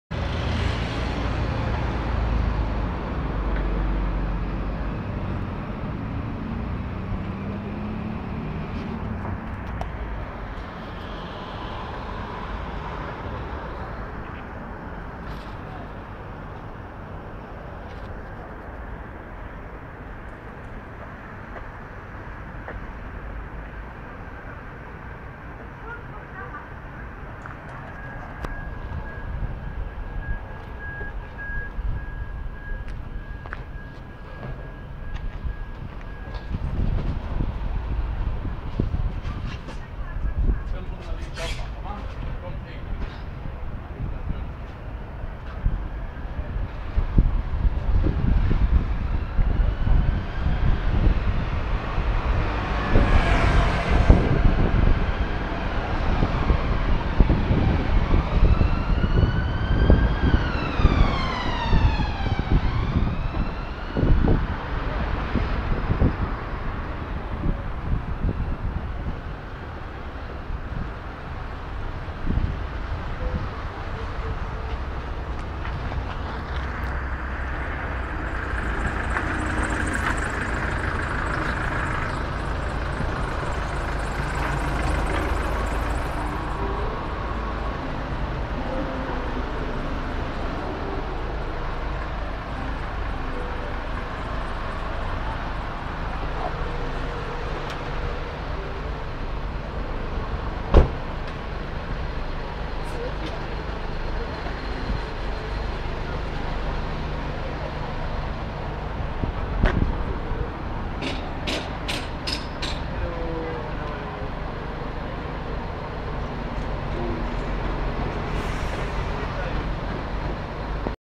FSA - 009 ... Daily Delivered Un-Refrigerated Fish ....
Daily Delivered Un-Refrigerated Fish ... alongside, dirty rubbish, a mop & bucket ... With no inspections by Camden's FSA Officials.  Description